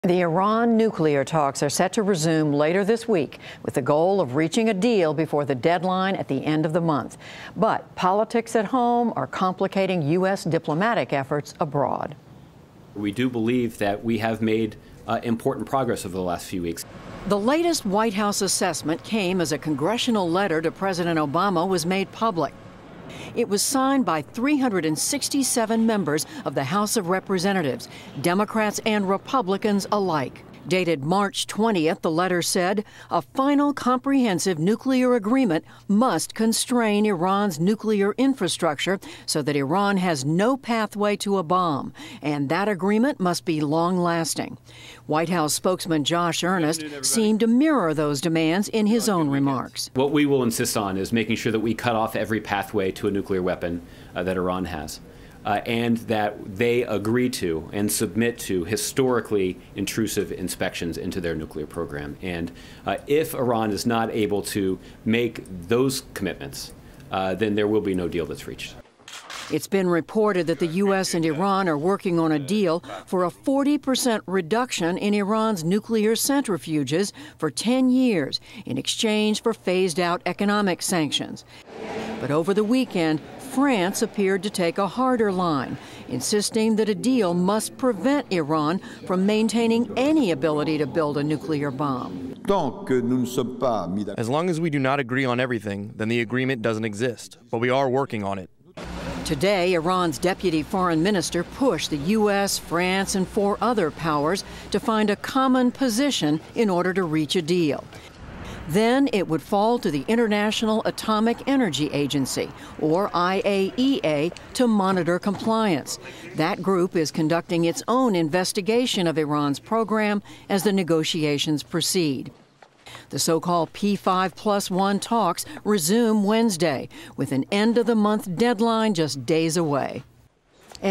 House to Obama: Iran must have ‘no pathway to a bomb’
Negotiations over Iran’s nuclear program are set to continue later this week, but politics at home may complicate discussions. After reports of a potential deal to reduce Iran’s nuclear centrifuges by 40 percent for 10 years, President Obama received a bipartisan letter signed by 367 members of the House insisting that Iran have “no pathway to a bomb.” Judy Woodruff rep